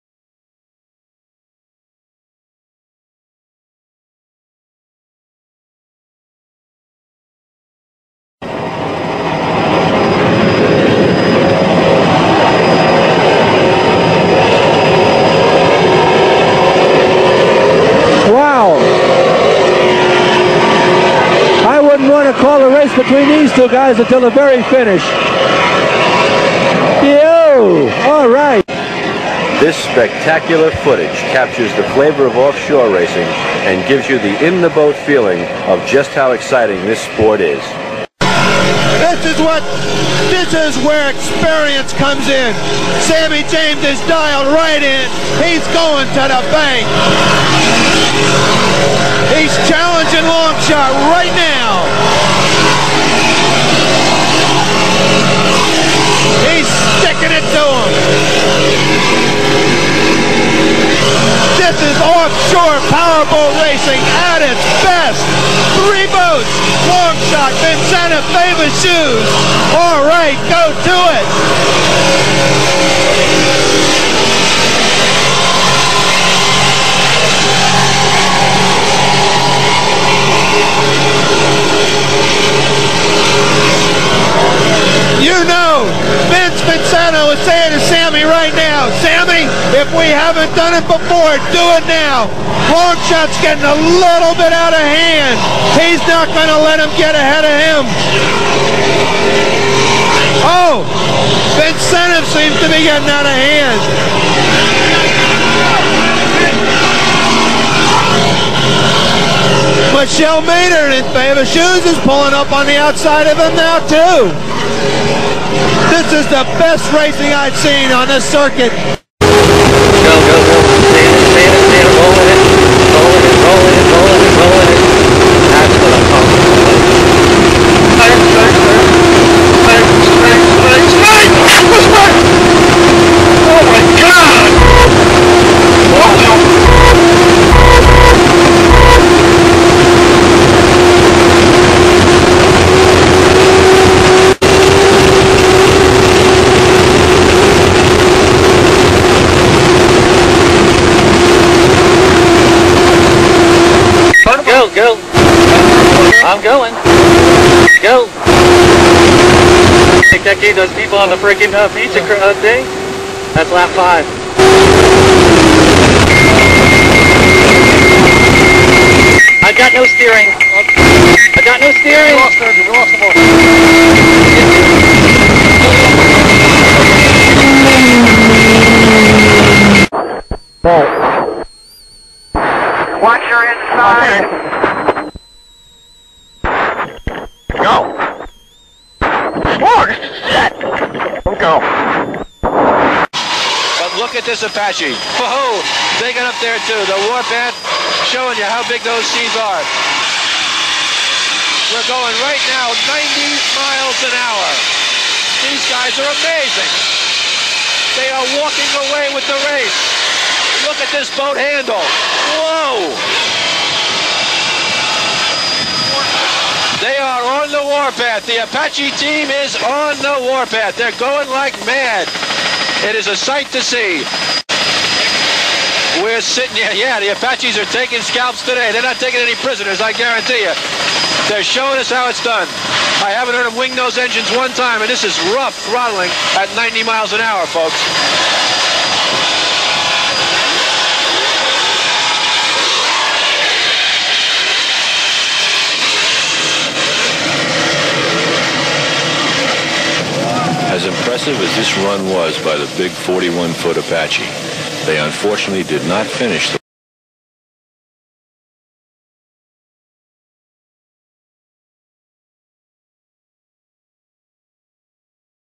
Offshore Powerboat Racing when boats were boats!
Short clips of offshore powerboats as they used to be but with a few newer type craft included! Speeds of the boats shown are from between 90 to 160 mph!